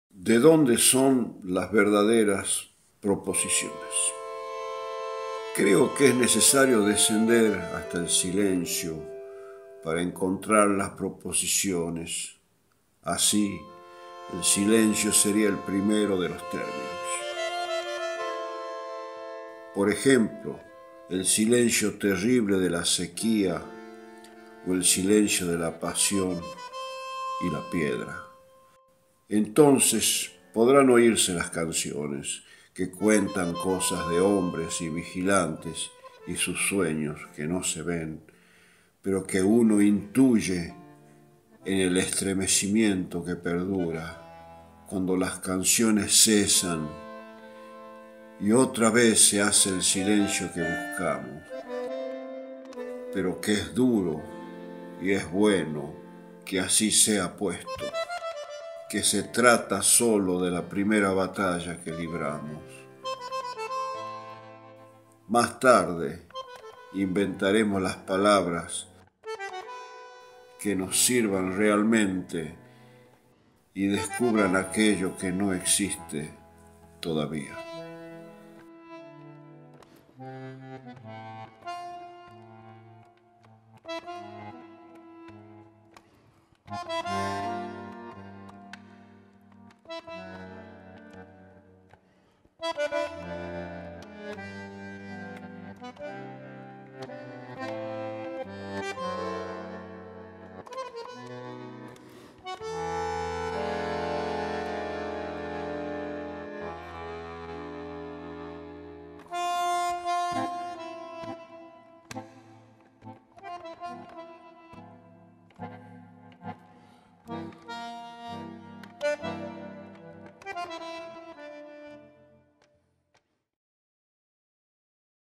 "De dónde son las verdaderas proposiciones", poema de José Coronel
El Archivo Nacional de la Memoria comparte la tercera edición de "El rocío en la piedra": escrituras, dibujos, voces, fotografías y movimientos, hilos de un tejido insistente, en memoria de poetas desaparecidas y desaparecidos por el terror de Estado.

En esta ocasión se presenta "De dónde son las verdaderas proposiciones", poema de José Coronel, militante y poeta, asesinado el 29 de septiembre de 1976 en Villa Luro, Buenos Aires.

Poema reunido en Coronel, José Carlos (2013) "Aquello que no existe todavía". Colección Los detectives salvajes. City Bell: De la Talita Dorada.